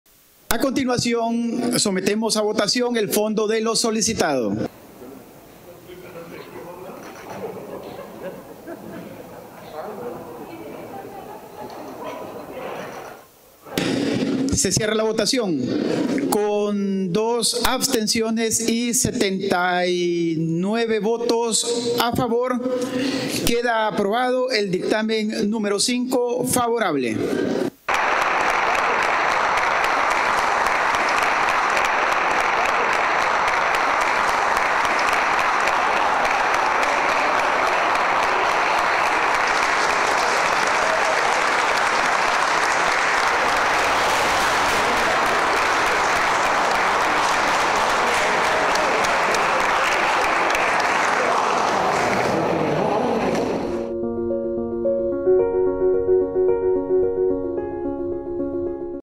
Asamblea Legislativa aprueba la Ley Nacer con Cariño
NacerConCariño 📹📺 || "Con dos abstenciones y 79 votos a favor queda aprobado el dictamen número cinco favorable", anunció el presidente de la Asamblea Legislativa, Ernesto Castro, sobre la aprobación de la Ley Nacer con Cariño.